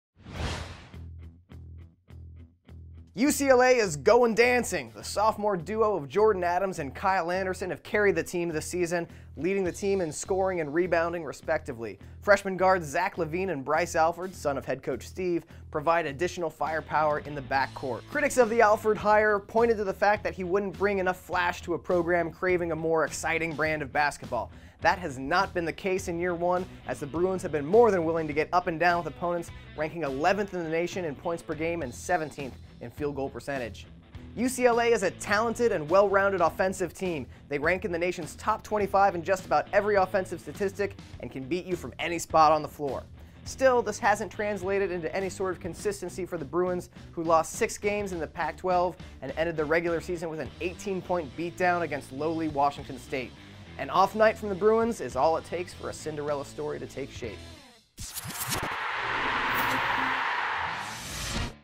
2014 NCAA Tournament: UCLA Bruins Team Profile
SB Nation is back with another year of Chasing Cinderella videos, including 20 breakdowns of higher-seeded teams that potential Cinderella squads look to upset in this year's Big Dance. In this installment, Matt Ufford explains what you can expect to see out of the UCLA Bruins.